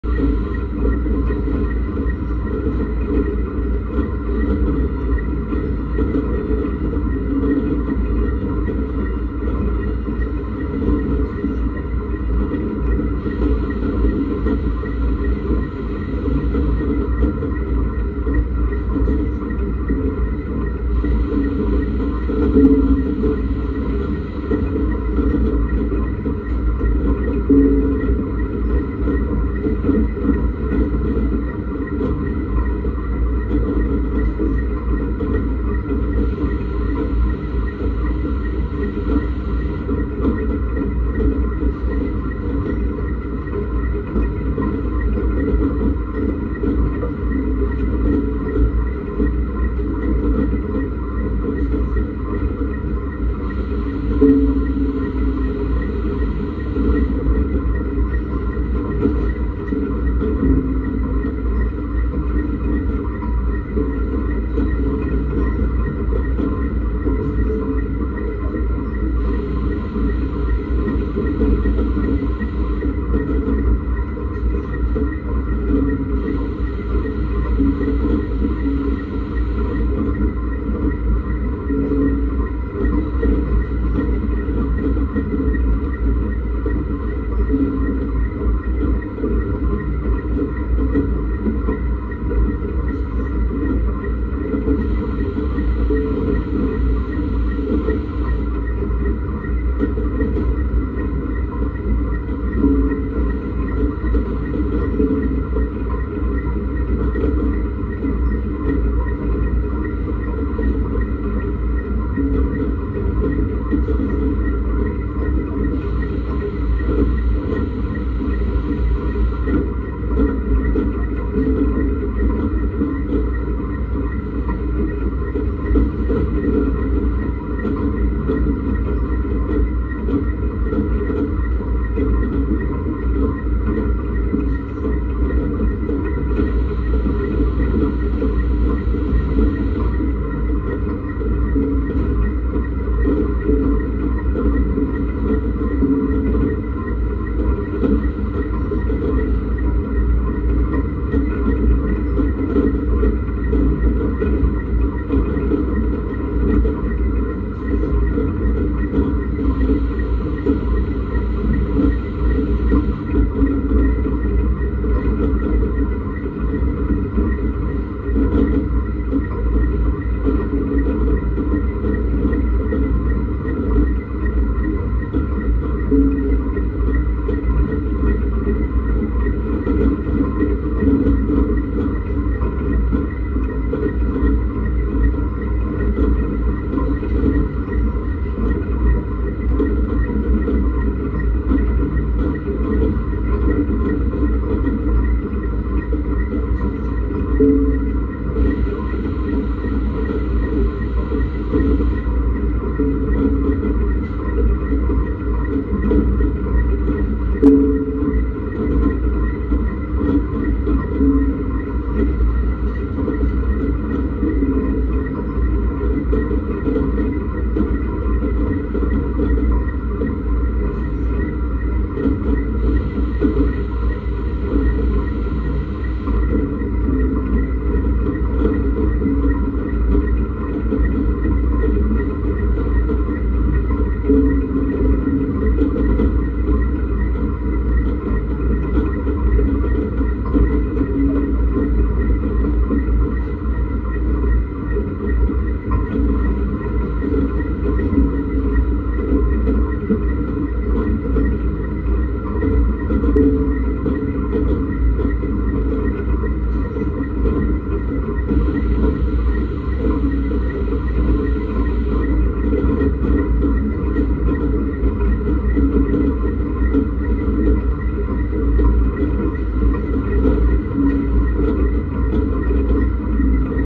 Grading potatoes
Grading potatoes and putting them into paper bags for market